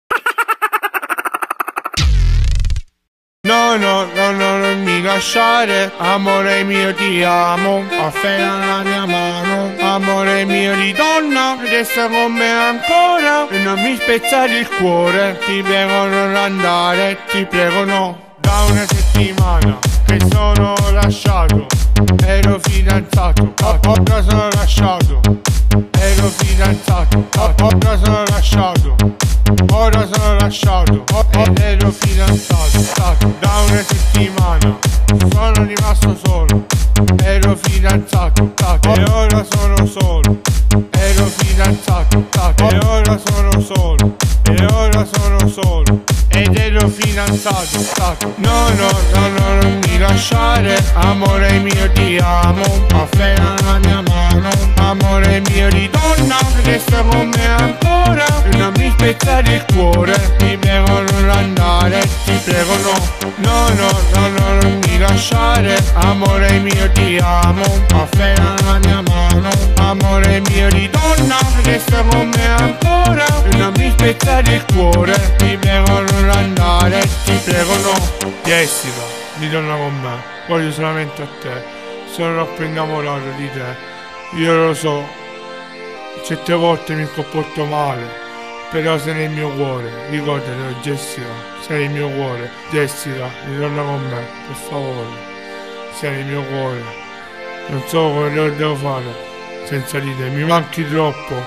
Non mi lasciare
Ahahahaha guardate quest uomo cosa si e ridotto a fare perché la sua ragazza lo ha lasciato